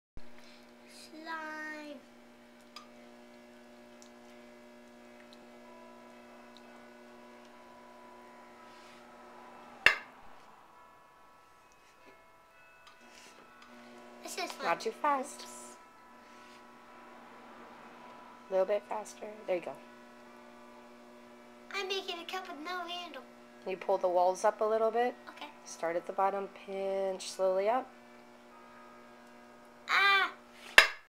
Fired Up kid 6 years old
My boys' first time  on the Potter's Wheel